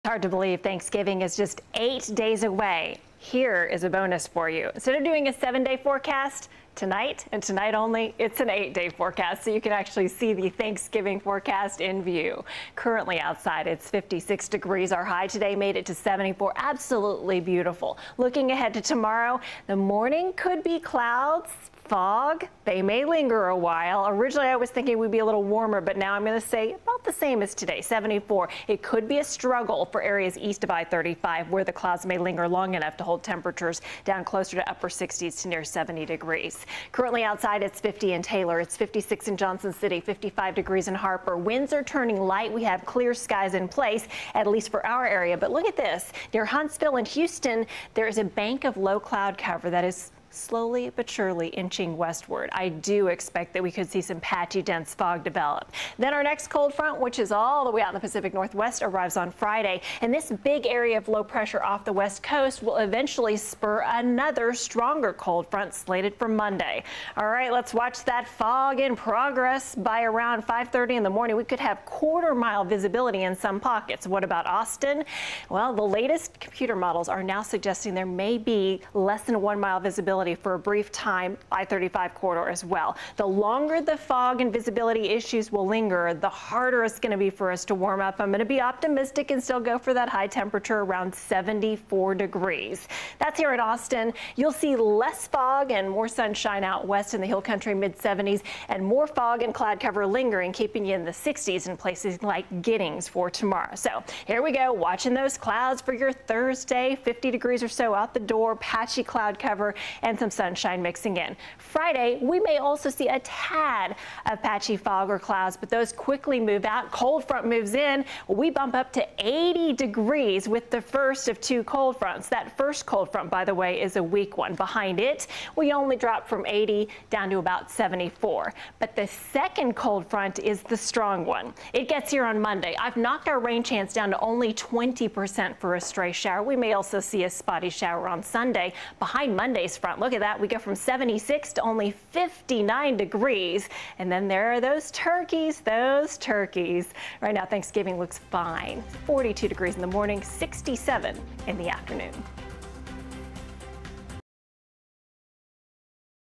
Cold front #1 Friday, cold front #2 Monday. ChiksClips - Wednesday, November 15th, 2023
Cold front #1 Friday, cold front #2 Monday. Cool and nice Thanksgiving! ChiksClips - Wednesday, November 15th, 2023